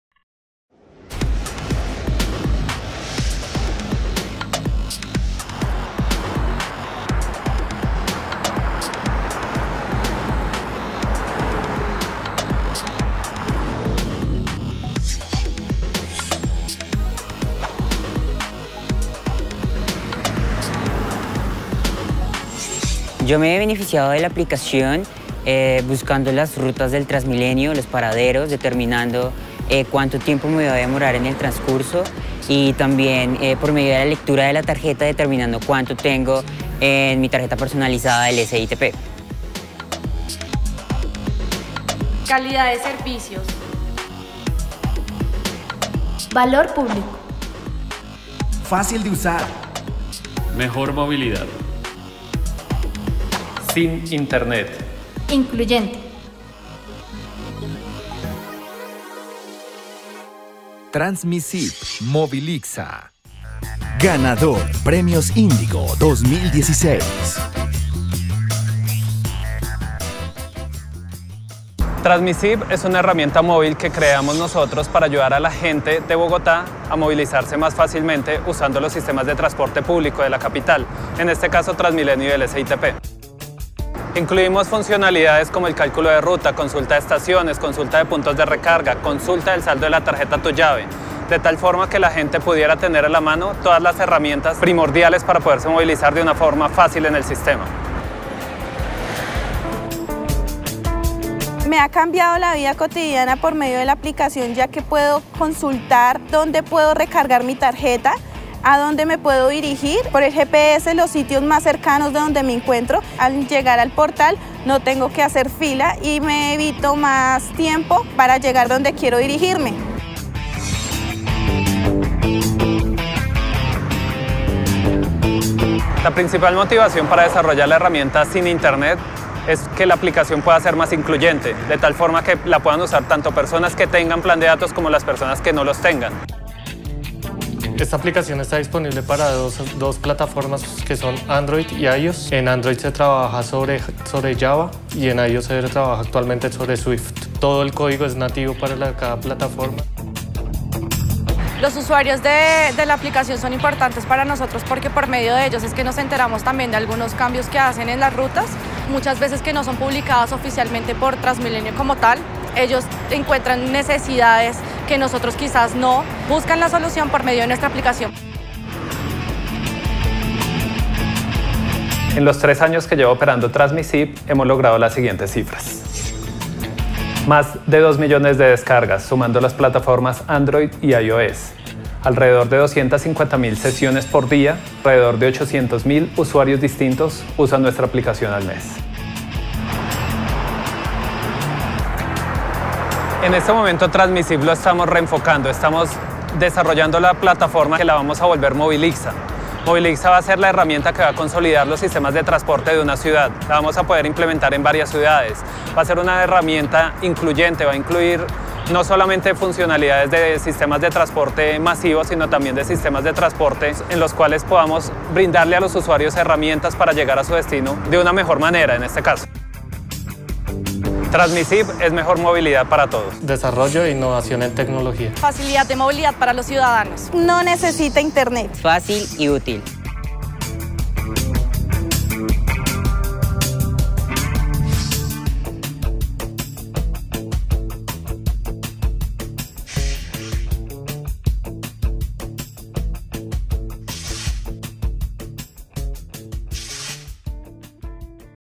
Transmisitp - Ganador Premios Indigo 2016 C29 N6
Transmisitp herramienta móvil que ayuda a los ciudadanos a movilizarse con facilidad en la ciudad.